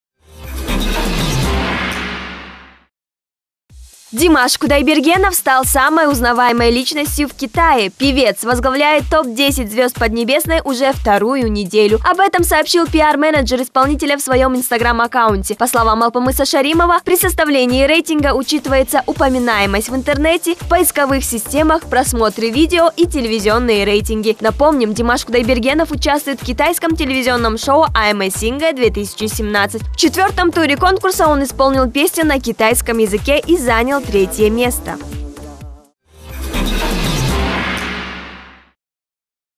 Димаш Кудайбергенов стал самой узнаваемой личностью в Китае!
Димаш Кудайбергенов стал самой узнаваемой личностью в Китае! Певец возглавляет топ десять звезд Поднебесной уже вторую неделю. Об этом сообщил пиар-менеджер исполнителя в своем инстаграм-аккаунте. По словам Алпамыса Шаримова при составлении рейтинга учитывается упоминаемость в интернете, в поисковых системах, просмотры видео и телевизионные рейтинги. Напомним, Димаш Кудайбергенов участвует в китайском телевизионном шоу «I am a singer - 2017». В четвертом туре конкурса он исполнил песню на китайском языке и занял третье место.